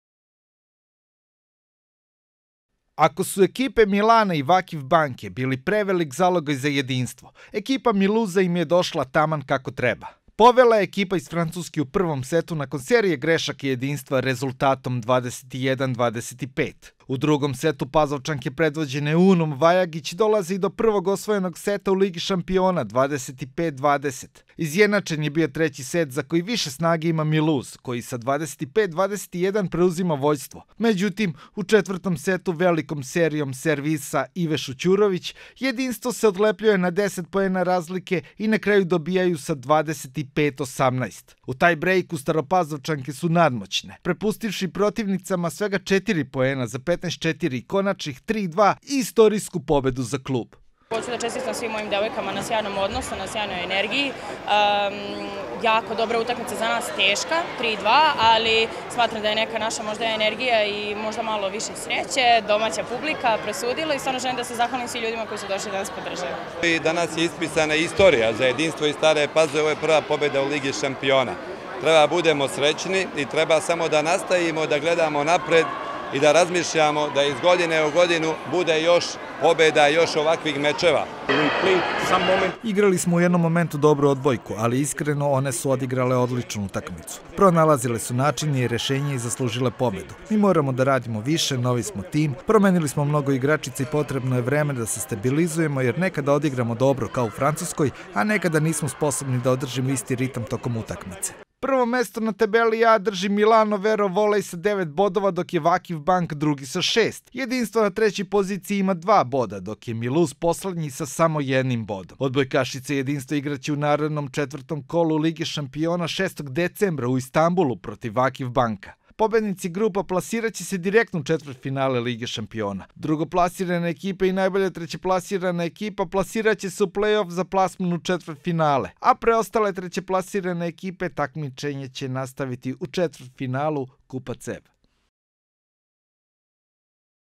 Jedinstvo savladalo Volej Miluz Alzas
Odbojkšice Jedinstva iz Stare Pazove sinoć su u Rumi u 3. kolu CEV Lige šampiona savladale ekipu Volej Miluz Alzas sa 3:2.